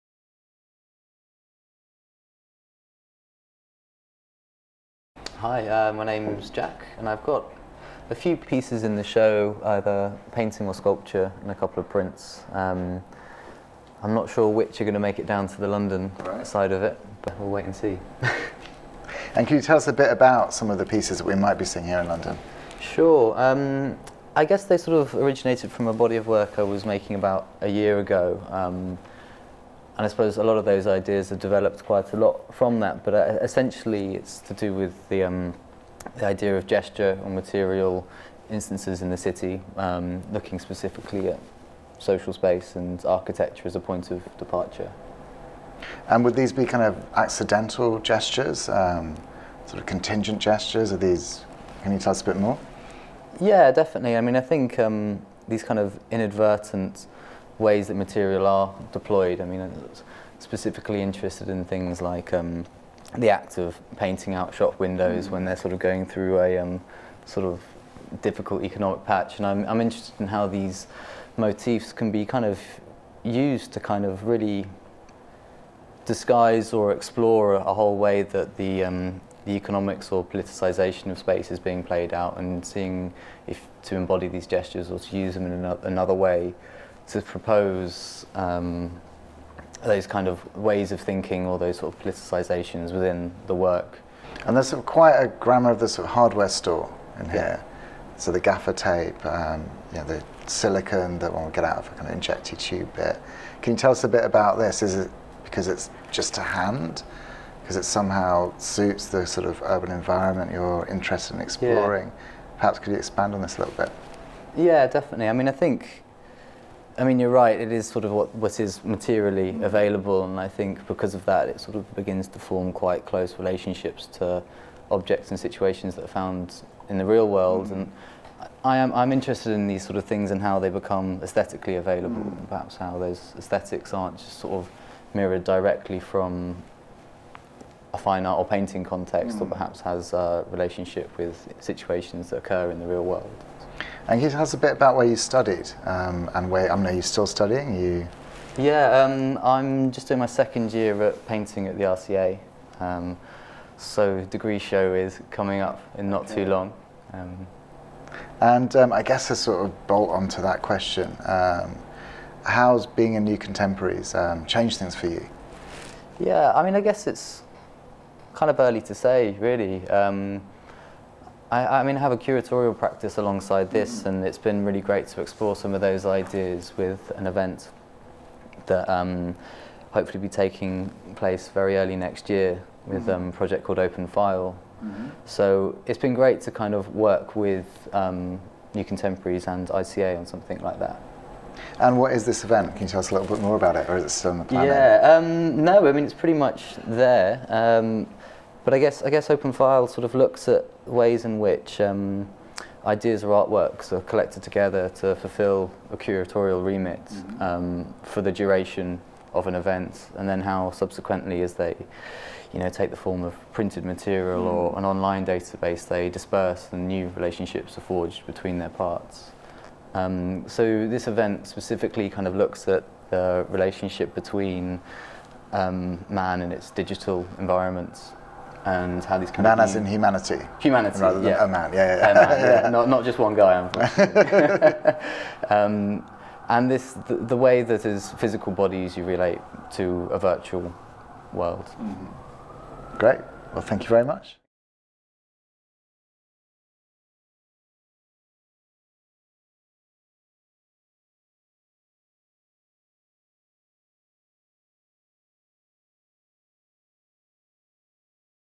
Bloomberg New Contemporaries 2012: Jack Brindley
Bloomberg New Contemporaries 2012:
Participant artist Jack Brindley talks to critic and writer Tom Morton about his work in the exhibition.